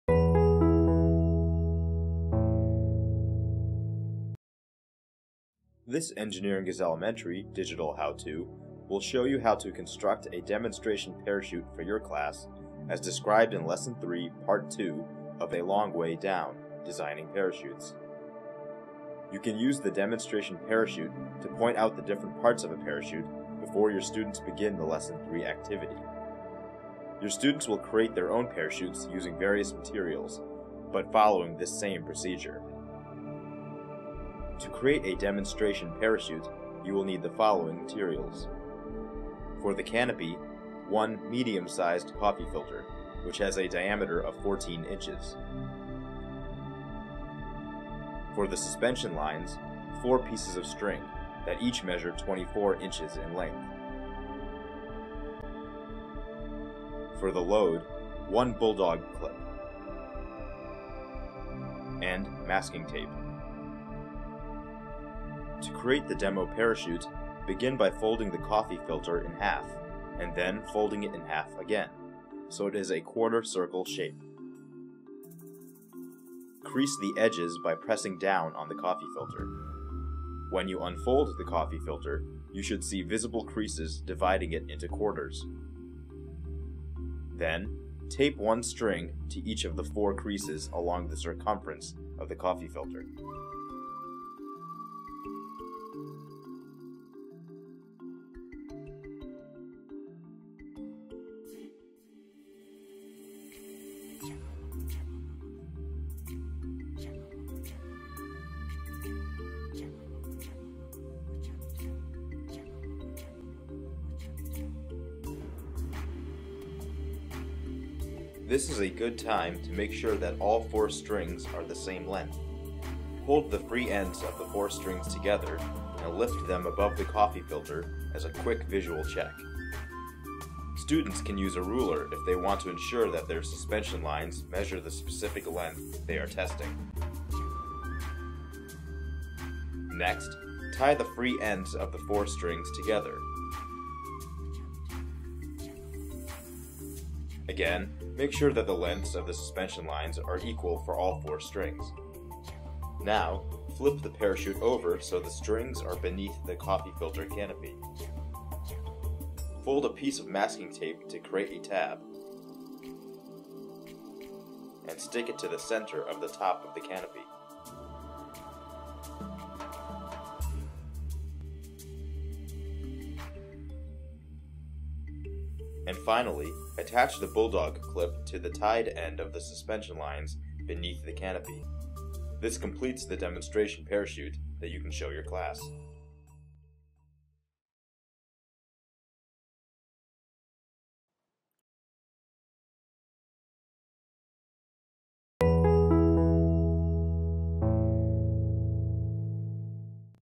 EiE How-To: Designing Parachutes - How to Construct a Demonstration Parachute
This video shows you how to make optional arm extenders from yardsticks so that students can drop their parachutes from a height of 7 feet while standing on the floor.